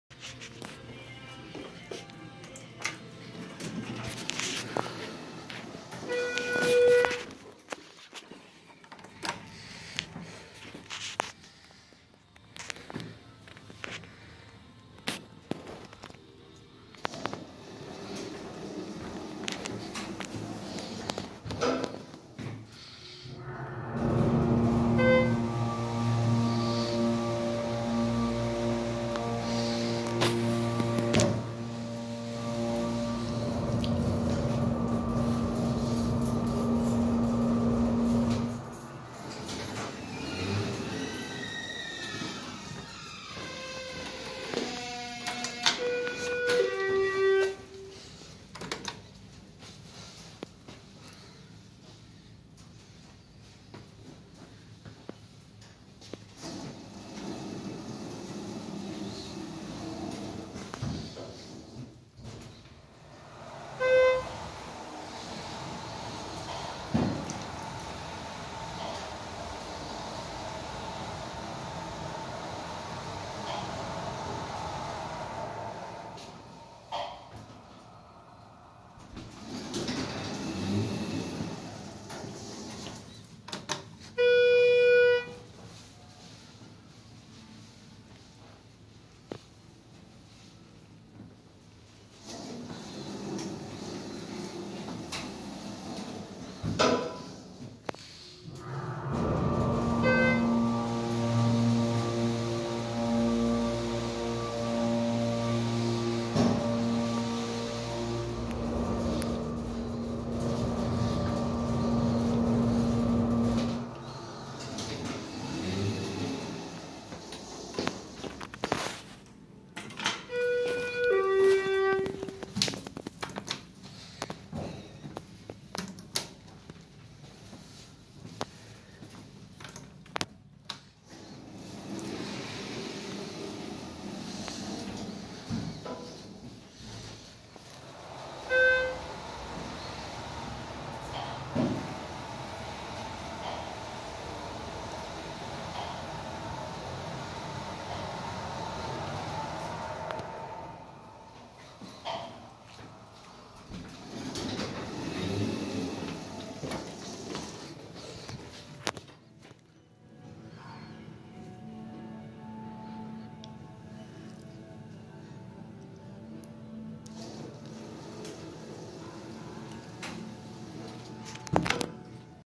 Schindler 330a Hydraulic Elevator Barnes And Noble Waukesha WI
Nice Schindler 330a Very Bright With The Lights And A Very Load Motor